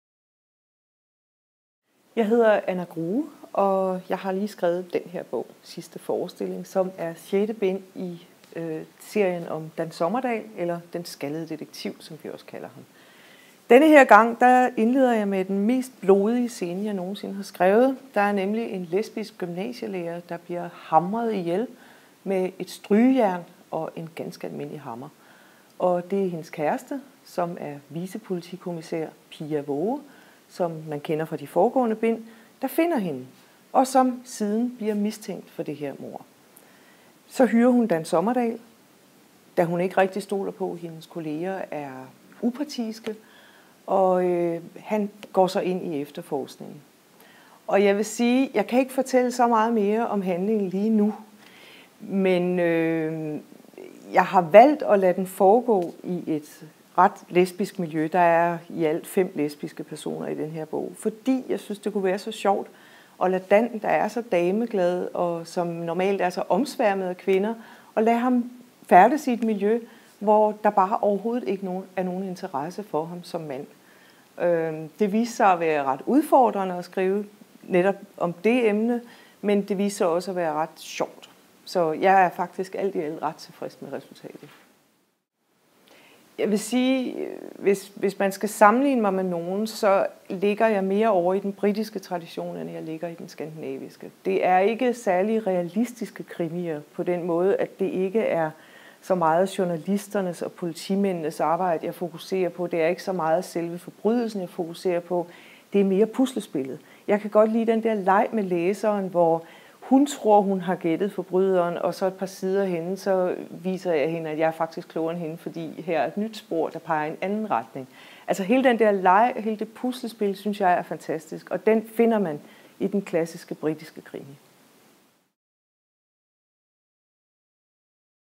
Mød Anna Grue på Krimimessen 2014
I "Sidste forestilling" lader Anna Grue en lesbisk kvinde blive hamret ihjel med et strygejern og en hammer. Du må selv læse bogen, hvis du vil vide hvem morderen er - så er du også godt forberedt til at møde Anna Grue på årets krimimesse.